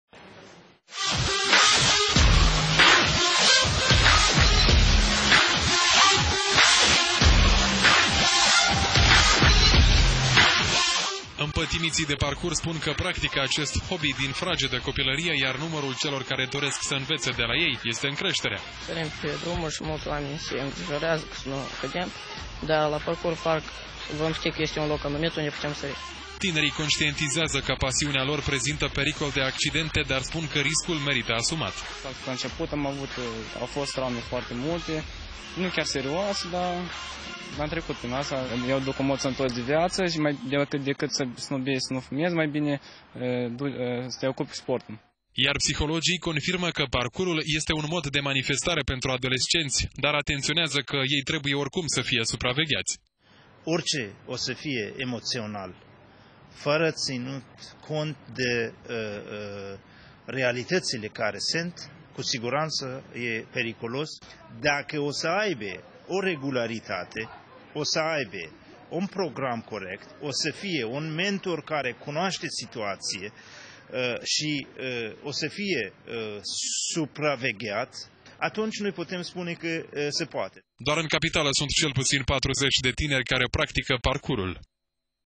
Amatorii de parkour din Chişinău îşi doresc un parc specializat.
Vrem Parkour Park)))))))